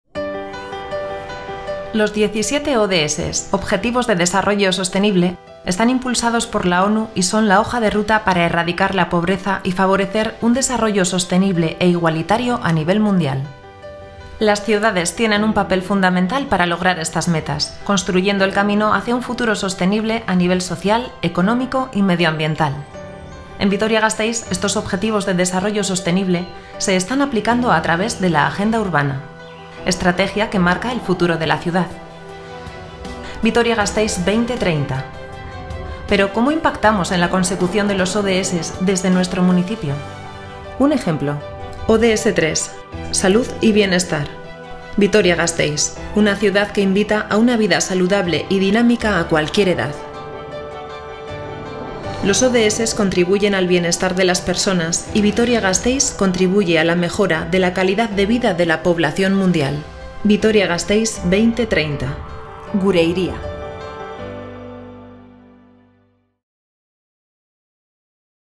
Salud y bienestar - ODS 3 en la ciudad – Agenda Vitoria-Gasteiz 2030
Los 17 ODS – OBJETIVOS DE DESARROLLO SOSTENIBLE están impulsados por la ONU y son la hoja de ruta para erradicar la pobreza y favorecer un desarrollo sostenible e igualitario a nivel mundial. En Vitoria-Gasteiz se están aplicando a través de la Agenda Urbana: Vitoria-Gasteiz 2030

¿Pero, cómo impactamos en la consecución de los ODS desde nuestro municipio?

UN EJEMPLO: ODS 3 Salud y bienestar

GARAPEN JASANGARRIRAKO 17 HELBURUAK bultzatzen ditu, ibilbide-orri bat osatzen dutenak mundu-mailan pobrezia desagerrarazteko eta berdintasunezko garapen jasangarri bat ahalbidetzeko. Gasteizen, Agendaren bitartez aplikatzen ari dira

Nolanahi ere, zer egiten dugu gure udalerritik, helburuak lortzeko bidean inpaktua eragiteko?

ADIBIDEZ: Hirugarren Helburua: Osasuna eta ongizatea.